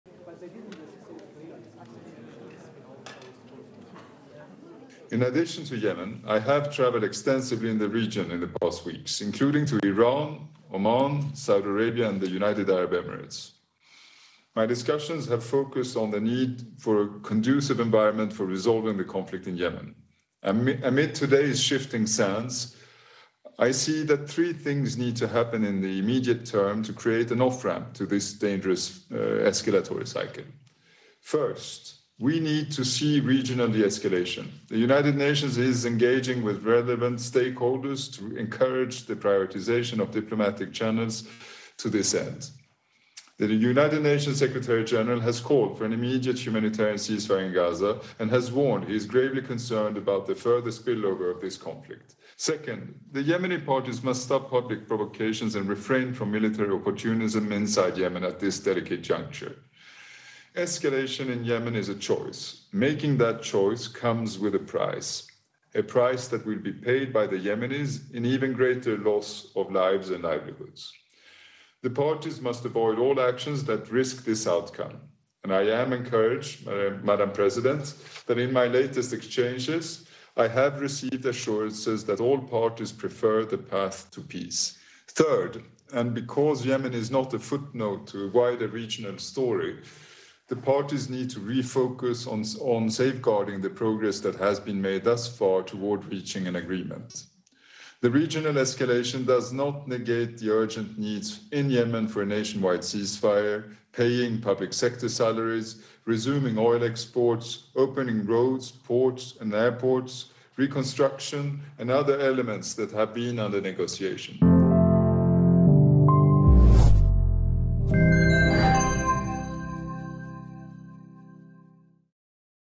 UN calls for ending 'dangerous escalatory cycle' in Yemenmp4
The United Nations' special envoy for Yemen calls for immediate action to end the "dangerous escalatory cycle" in the war-torn country, particularly given recent attacks by Huthi rebels in the Red Sea.